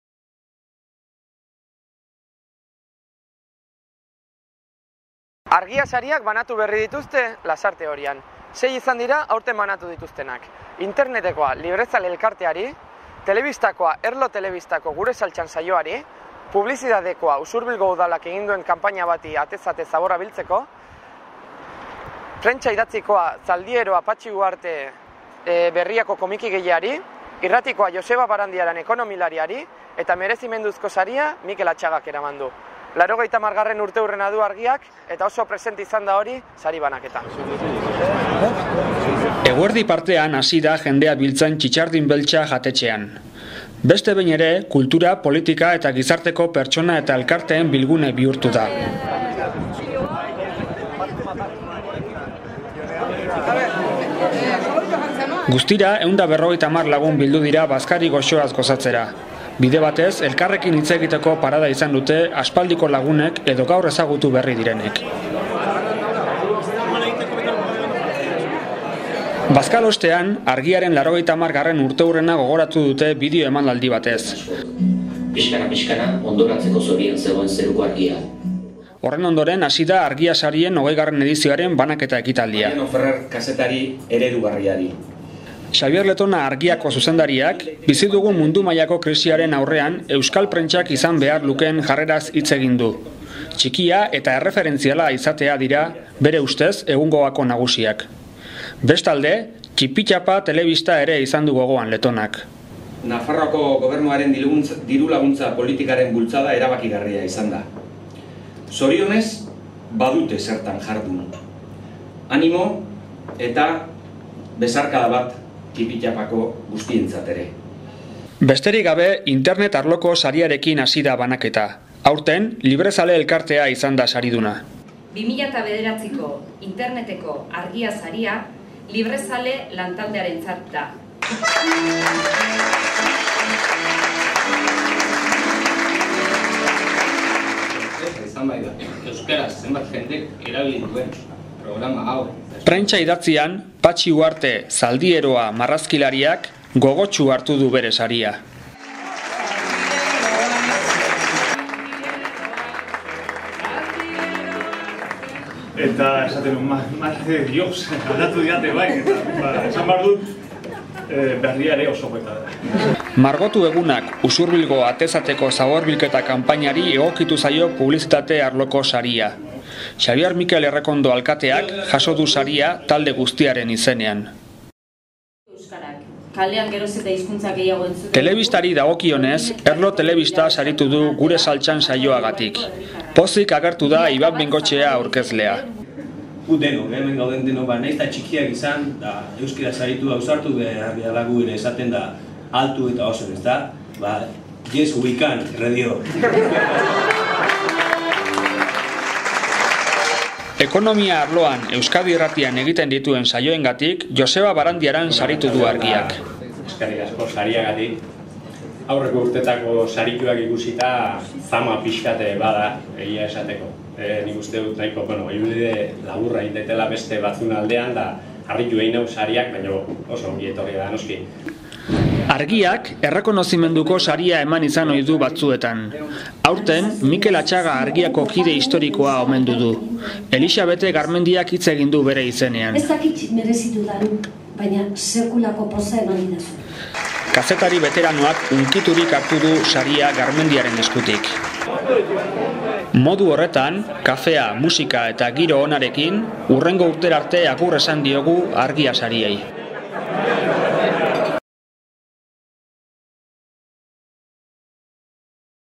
Argia Sariak 2009
Argia Sariak 2009. Librezale (internet saria), Patxi Huarte 'Zaldi Eroa' (prentsa saria), Margotu egunak atez-ateko zabor bilketa sustatzeko Usurbilgo udalak egin duen kanpaina (publizitate saria), Erlo Telebista (telebista saria) eta Mikel Atxaga (Merezimenduzko saria)